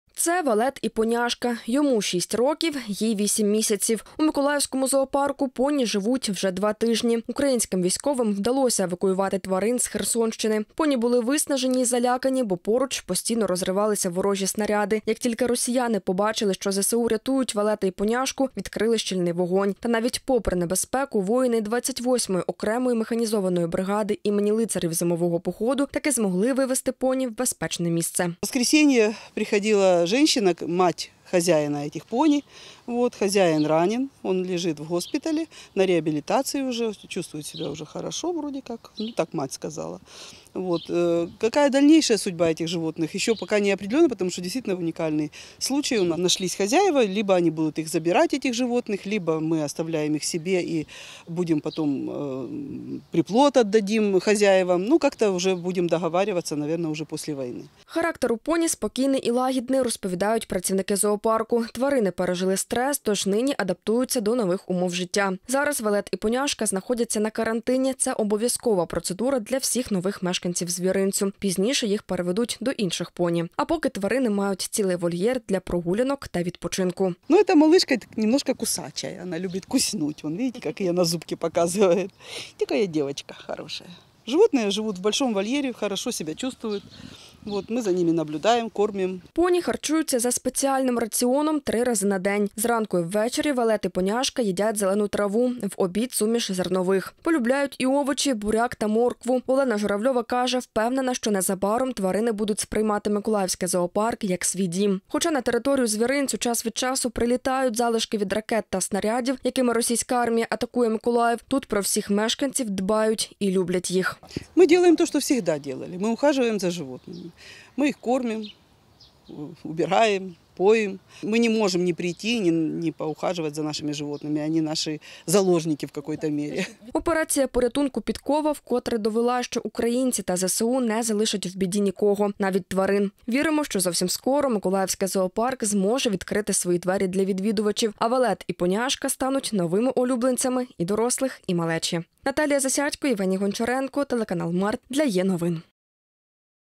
Українські військові евакуювали з Херсонщини двох поні. Тепер вони живуть в миколаївському зоопарку
Операція «Підкова». Воїни 28-ї окремої механізованої бригади імені Лицарів Зимового Походу евакуювали з села Посад-Покровське, що на тимчасово окупованій Херсонщині, двох поні. Нині тварини знаходяться в Миколаївському зоопарку і поки будуть жити в звіринці. Як себе почувають, дивіться у сюжеті.